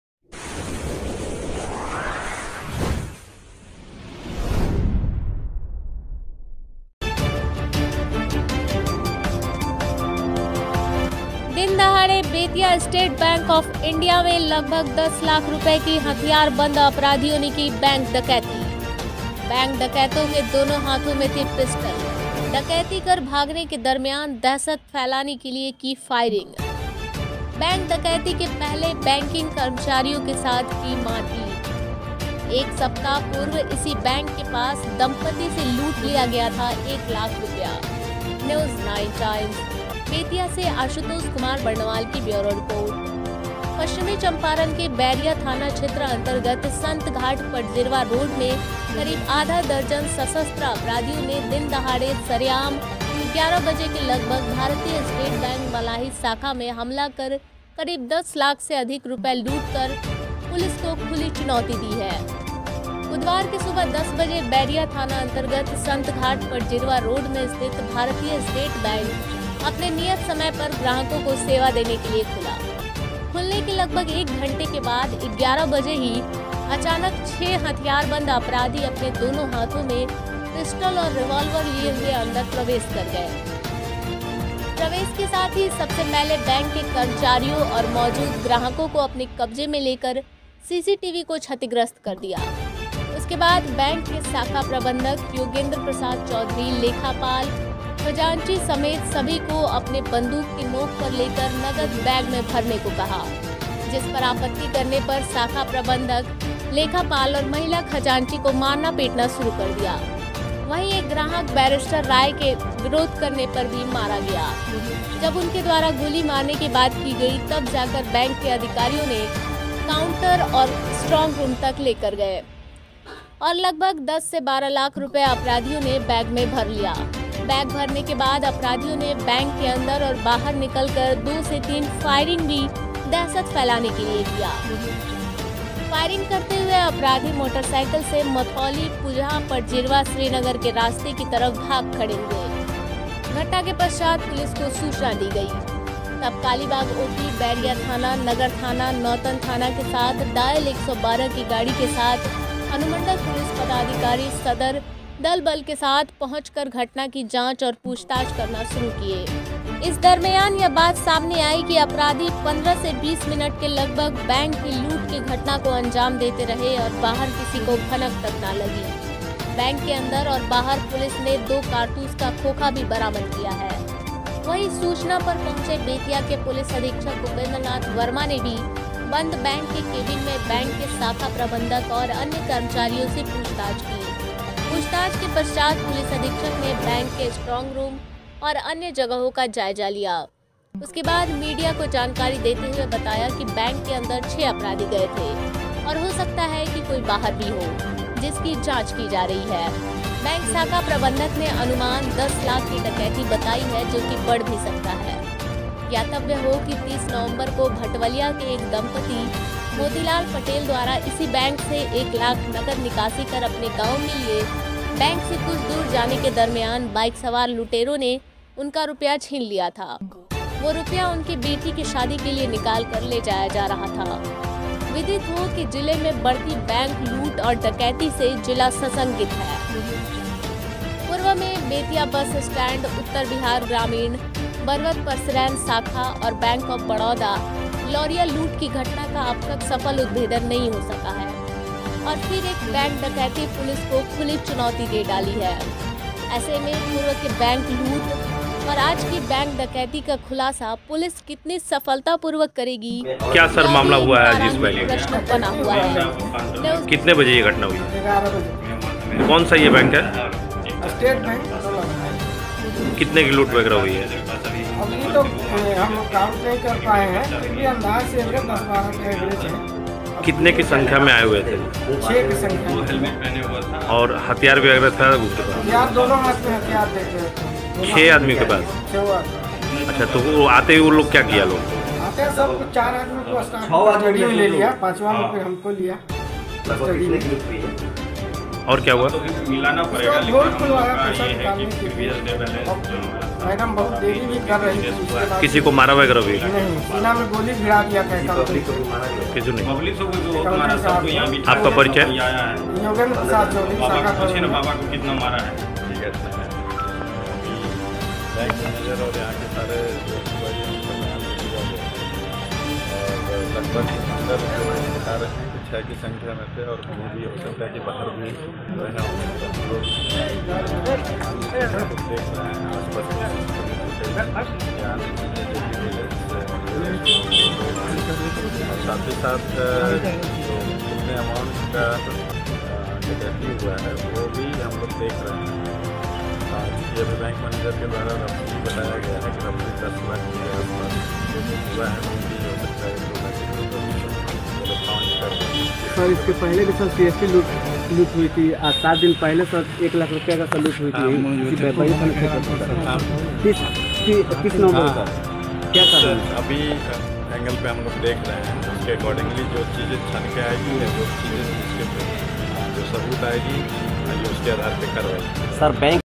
दिनदहाड़े बेतिया स्टेट बैंक ऑफ इंडिया में लगभग 10 लाख रूपया की हथियारबंद अपराधियों ने की बैंक डकैती
बैंक डकैतों के दोनों हाथों में थी पिस्टल, डकैती कर भागने के दरम्यान दहशत फैलाने के लिए की फायरिंग

बैंक डकैती के पहले बैंकिंग कर्मचारियों के साथ मारपीट

एक सप्ताह पूर्व इसी बैंक के पास दम्पत्ति से लूट लिया गया था एक लाख रूपया

न्यूज9 टाइम्स बेतिया से आशुतोष कुमार बरनवाल की ब्यूरो रिपोर्ट :-

पश्चिम चम्पारण के बैरिया थाना क्षेत्र अंतर्गत संत घाट पटजिरवा रोड में करीब आधा दर्जन सशस्त्र अपराधियों ने दिनदहाड़े सरेआम 11 बजे के लगभग भारतीय स्टेट बैंक मलाही शाखा में हमला कर करीब 10 लाख से अधिक रुपया लूट कर पुलिस को खुली चुनौती दिया है।

बुधवार के सुबह 10 बजे बैरिया थानांतर्गत संत घाट पटजिरवा रोड में स्थित भारतीय स्टेट बैंक अपने नियत समय पर ग्राहकों को सेवा देने के लिए खुला। खुलने के लगभग 1 घंटा के बाद 11 बजे ही अचानक 6 हथियारबंद अपराधी अपने दोनों हाथों में पिस्टल/रिवाल्वर लिए हुए अंदर प्रवेश कर गए। प्रवेश के साथ ही सबसे पहले बैंक के कर्मचारियों और मौजूद ग्राहकों को अपने कब्जे में लेकर सीसीटीवी को क्षतिग्रस्त कर दिया। उसके बाद बैंक के शाखा प्रबंधक योगेन्द्र प्रसाद चौधरी, लेखापाल, खजांची समेत सभी को अपने बंदूक की नोंक पर लेकर नगद बैग में भरने को कहा। जिसपर आपत्ति करने पर शाखा प्रबंधक, लेखापाल और महिला खजांची को मारना पिटना शुरू कर दिया। वहीं एक ग्राहक बैरिस्टर राय के भी विरोध करने पर मारा गया। जब उनके द्वारा गोली मारने की बात कही गई तब जाकर बैंक के अधिकारियों ने काउंटर और स्ट्रांग रूम तक ले गए और लगभग 10 से 12 लाख रूपया अपराधियों ने बैग में भर लिया। बैग भरने के बाद अपराधियों ने बैंक के अंदर और बाहर निकल कर दो से तीन फायरिंग भी दहशत फैलाने के लिए किया। फायरिंग करते हुए अपराधी मोटरसाइकिल से मथौली पूजहां पटजिरवा, श्रीनगर के रास्ते की तरफ भाग खड़े हुए।

घटना के पश्चात पुलिस को सूचना दी गई तब कालीबाग ओपी, बैरिया थाना, नगर थाना, नौतन थाना के साथ डायल 112 की गाड़ी के साथ अनुमंडल पुलिस पदाधिकारी सदर दल बल के साथ पहुंच कर घटना की जांच और पूछ ताछ करना शुरू किए। इस दरम्यान यह बात सामने आई कि अपराधी 15 से 20 मिनट के लगभग बैंक की लूट को अंजाम देते रहें और बाहर किसी को भनक तक नहीं लगी। बैंक के अंदर और बाहर पुलिस ने दो कारतूस का खोखा भी बरामद किया है।

वहीं सूचना पर पहुंचे बेतिया के पुलिस अधीक्षक उपेन्द्रनाथ वर्मा ने भी बंद बैंक के केबिन में बैंक के शाखा प्रबंधक और अन्य कर्मचारियों से पूछताछ की। पूछताछ के पश्चात पुलिस अधीक्षक ने बैंक के स्ट्रांग रूम और अन्य जगहों का जायजा लिया। उसके बाद मीडिया को जानकारी देते हुए बताया कि बैंक के अंदर 6 अपराधी गए थे और हो सकता है कि बाहर भी हो, जिसकी जांच की जा रही है। बैंक शाखा प्रबंधक ने अनुमान 10 लाख की डकैती बताई है जो कि बढ़ भी सकता है।

ज्ञातव्य हो कि 30 नवम्बर को भटवलिया के एक दम्पत्ति मोतीलाल पटेल द्वारा इसी बैंक से एक लाख नगद निकासी कर अपने गांव के लिए बैंक से कुछ दूर जाने के दरम्यान बाइकसवार लूटेरों ने उनका रूपया छिन लिया था। वो रुपया उनकी बेटी के शादि के लिए निकाल कर ले जाया जा रहा था।

विदित हो कि जिले में बढ़ती बैंक लूट व डकैती से जिला सशंकित है। पूर्व में बेतिया बस स्टैंड उत्तर बिहार ग्रामीण, बरवत पसरैन शाखा और बैंक आॅफ बड़ौदा, लौरिया लूट की घटना का अब तक सफल उद्भेदन नहीं हो सका है और फिर एक बैंक डकैती पुलिस को खुली चुनौती दे डाली है। ऐसे में पूर्व के बैंक लूट और आज के बैंक डकैती का खुलासा पुलिस कितना सफलतापूर्वक करेगी, यह भी एक तारांकित प्रश्न बना हुआ है।news9times